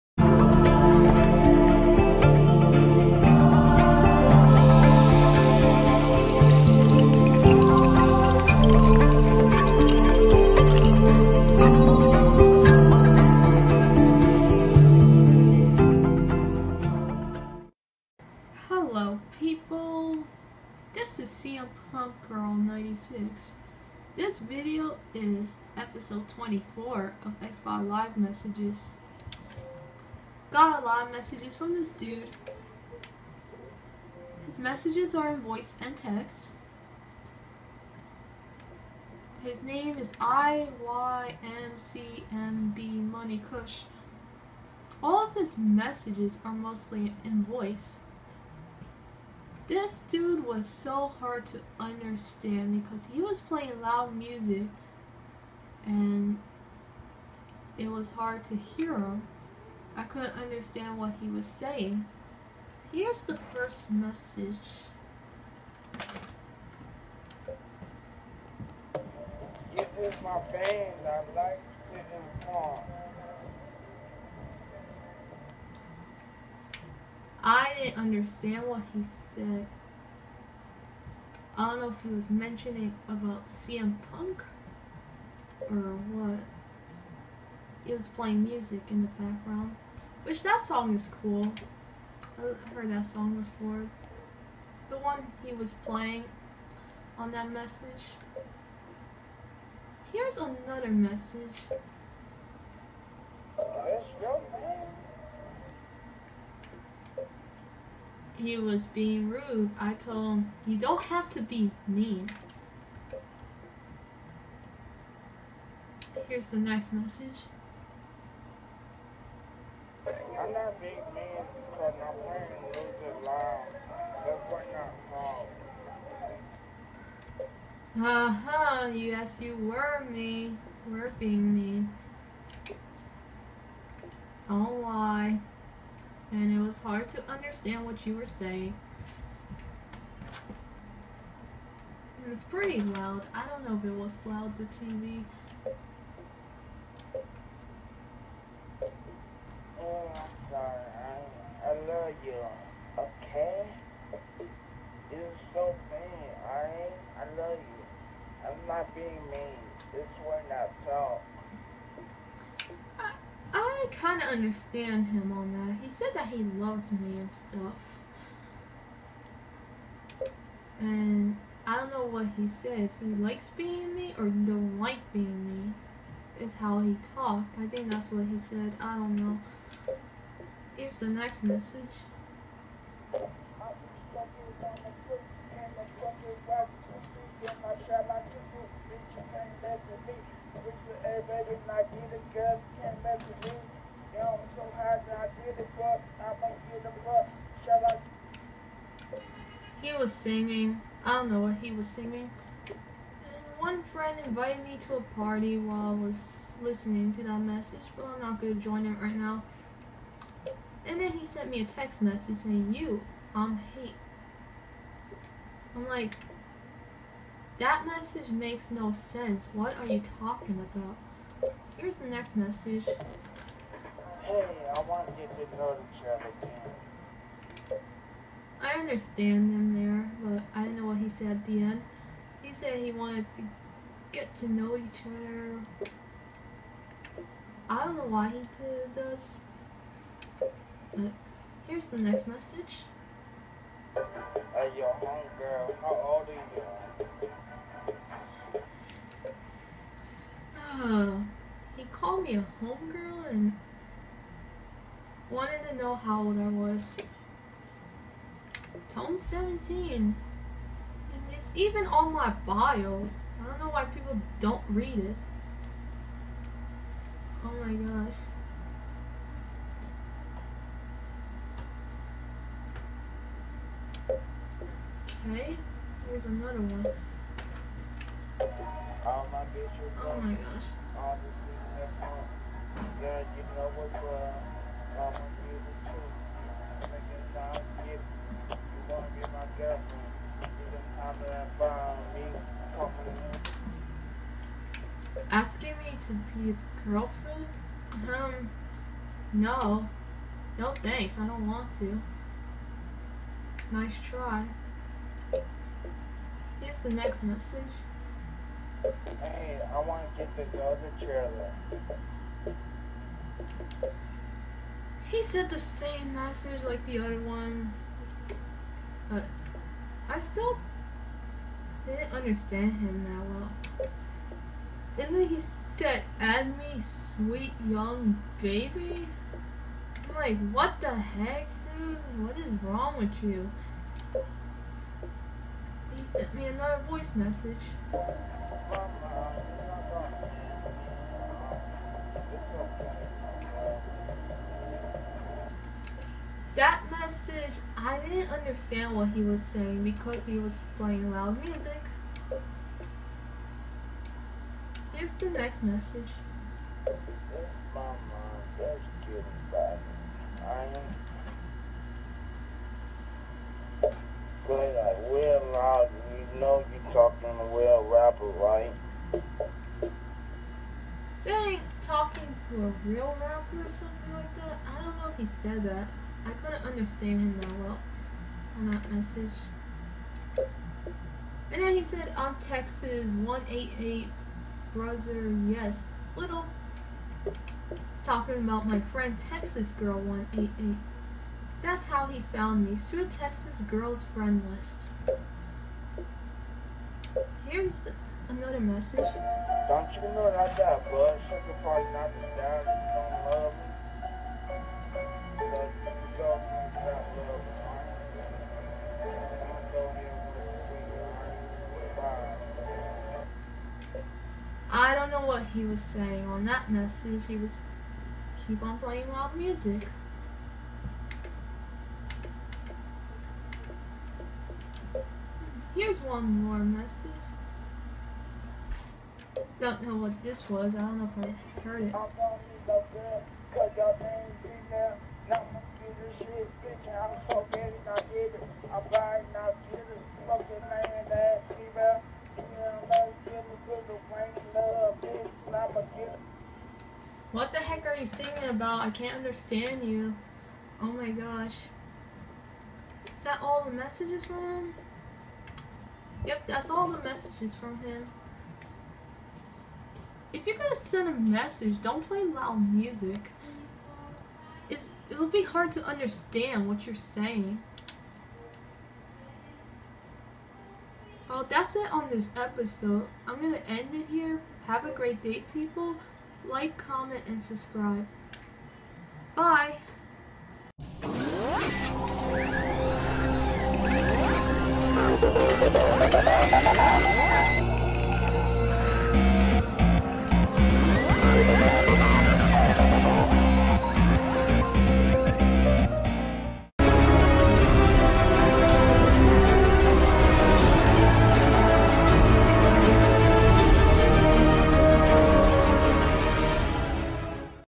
Xbox Live Messages Episode 24: It's Hard to Understand This Dude
This video is episode 24 of Xbox Live Messages. I got a lot of messages from this dude. His gamertag is IYMCMBMoneyKush. Most of the messages he sent me, I didn't know what he was saying. It was hard to understand.